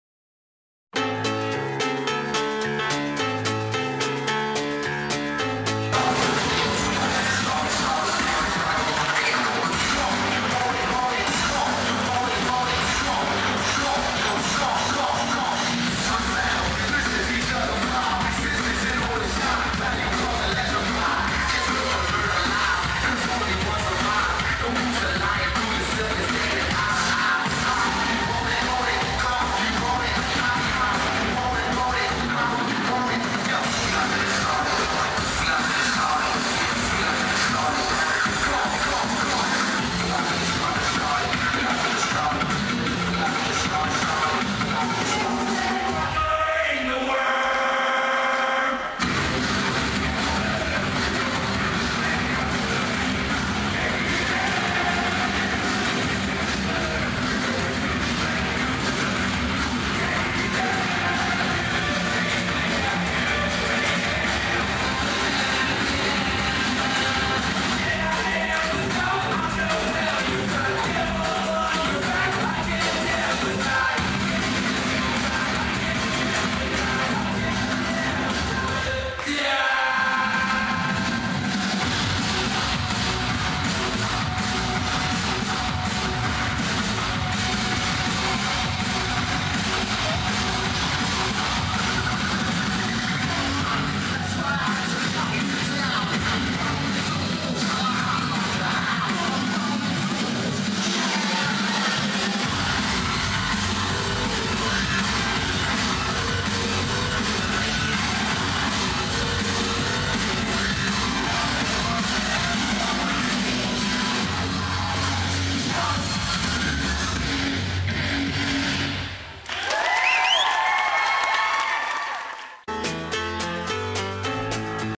Exhibice sport. aerobik - Chodov Cup 2011
Záznam exhibičního vystoupení sportovního aerobiku na mezinárodním závodu v moderní gymnastice Chodov Cup 2011 konaném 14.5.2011